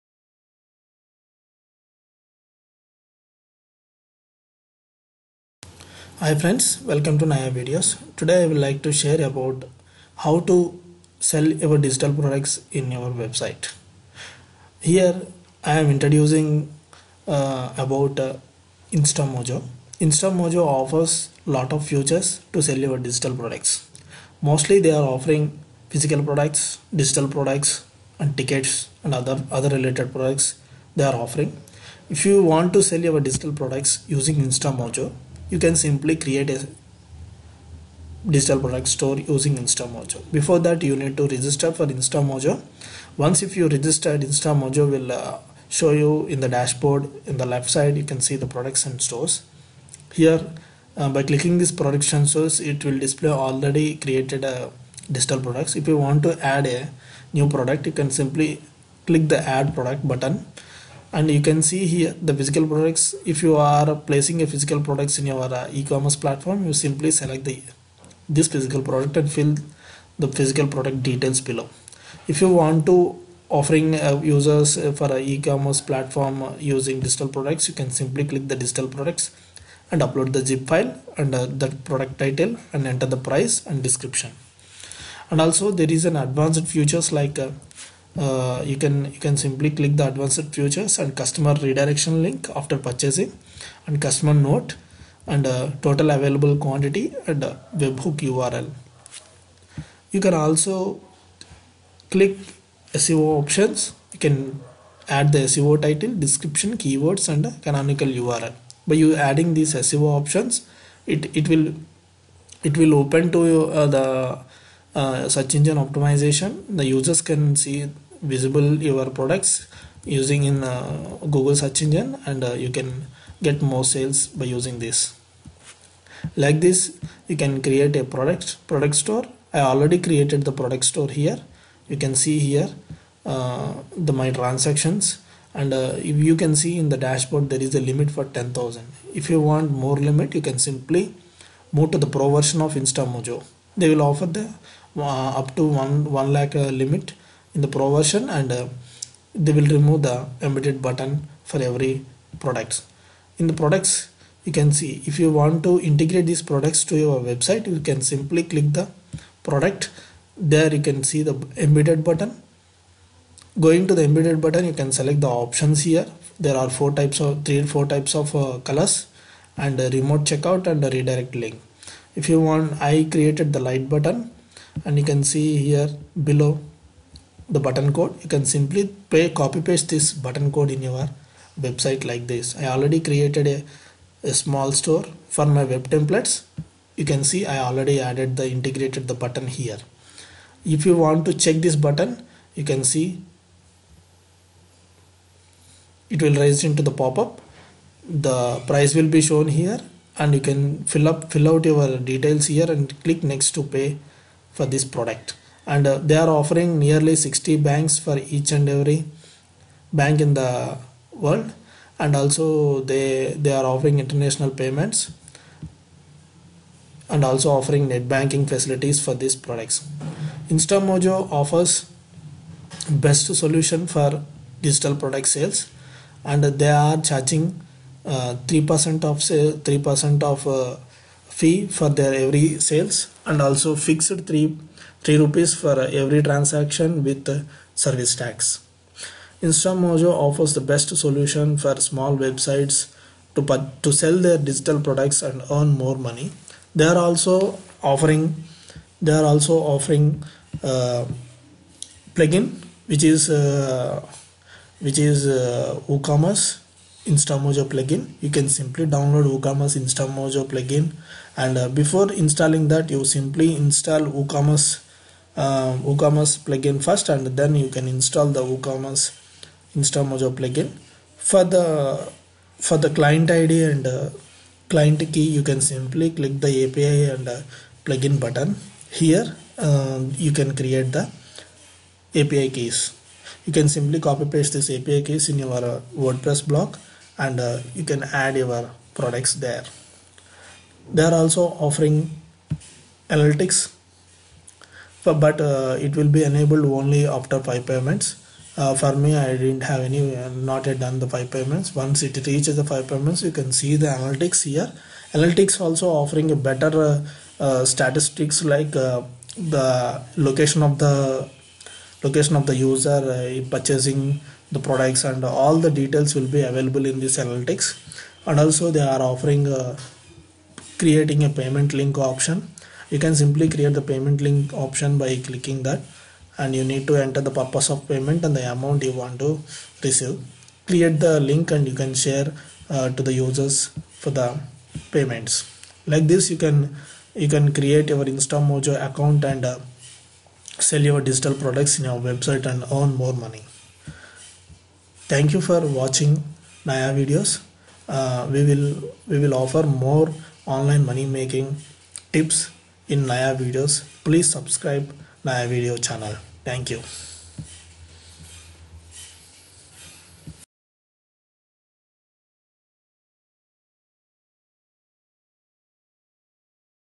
How to add Instamojo In Your Website for Payment Getaway | Instamojo Payment Gateway
You will learn in this video how to create an Instamojo payment gateway account with the Instamojo features offered. Instamojo is a free payment collection gateway with a free e-commerce store. If you are a new seller and looking for a payment gateway for Facebook or WhatsApp or other platforms, Instamojo is a better platform to collect payment and sell online.